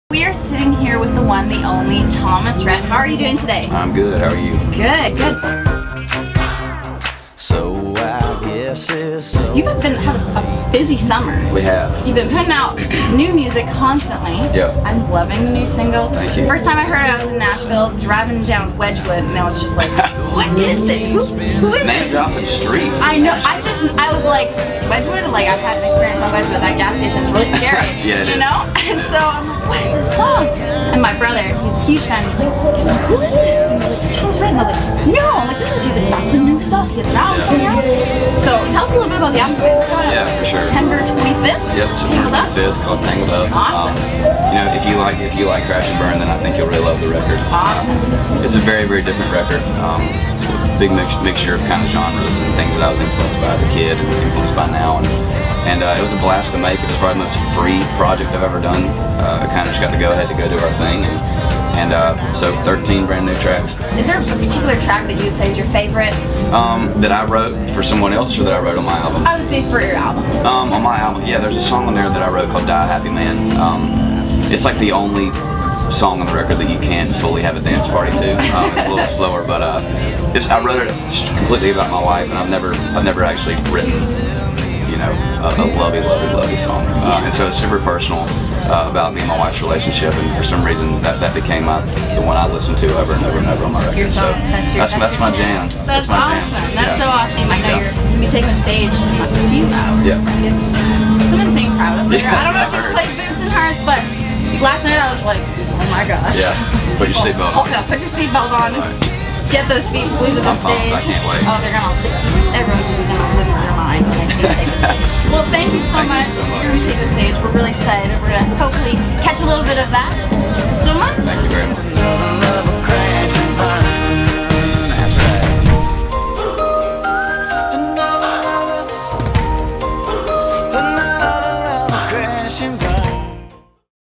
Thomas Rhett at Boots and Hearts 2015
Our Country Voice correspondent Livy Jeanne interviews Thomas Rhett